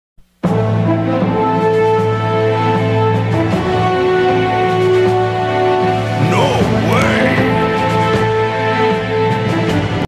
x meet the bat
X is a big fan of the batman and met him one day
made with anime studio 7